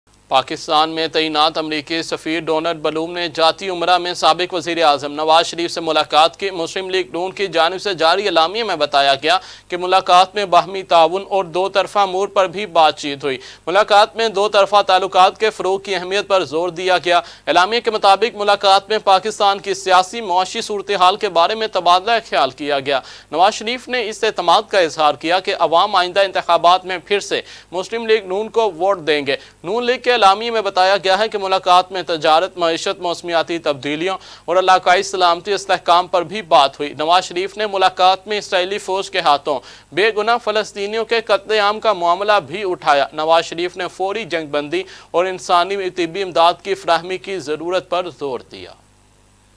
Nawaz Sharif meeting / important meeting / ex pm pakistan meeting / nawaz sharif / pmln / america/
American Ambassador Donald Bloom met former Prime Minister Nawaz Sharif in Jati Amra